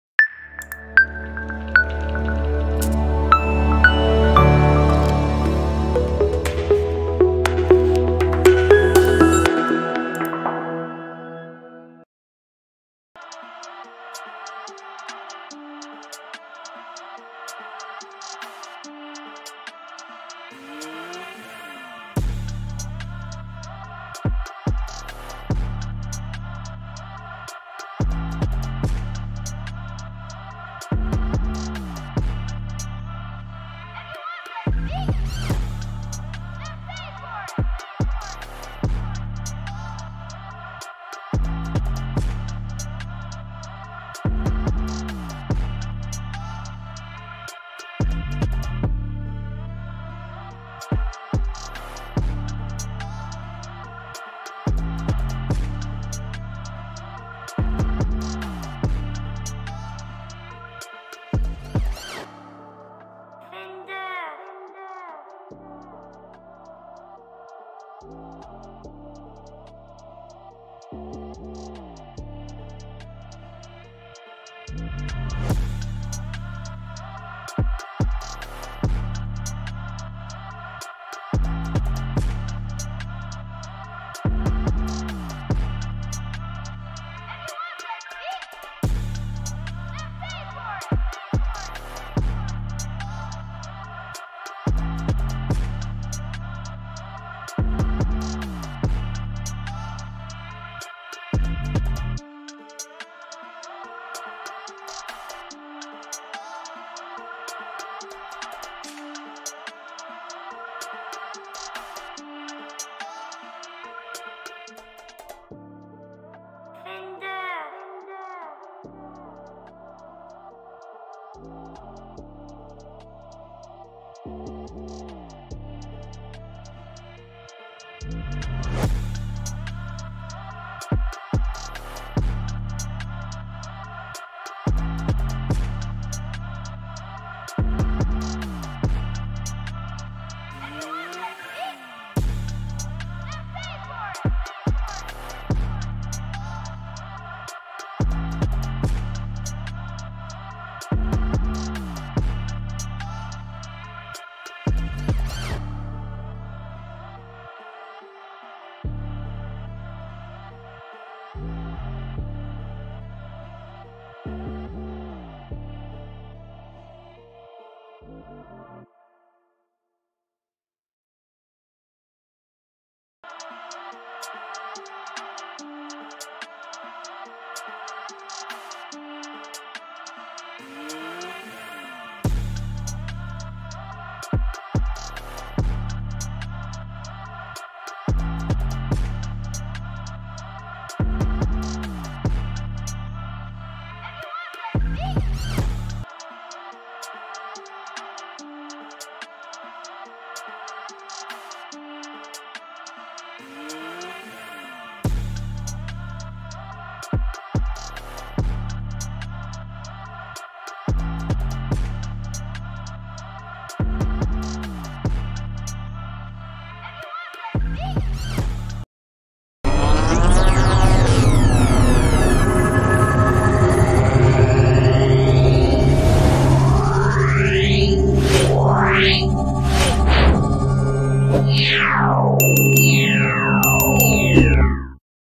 How to implement webview using jetpack compose |Android Studio |kotlin
How,when,what to use webview for. the web view feature help user to browse on a web application without launching it with a browser.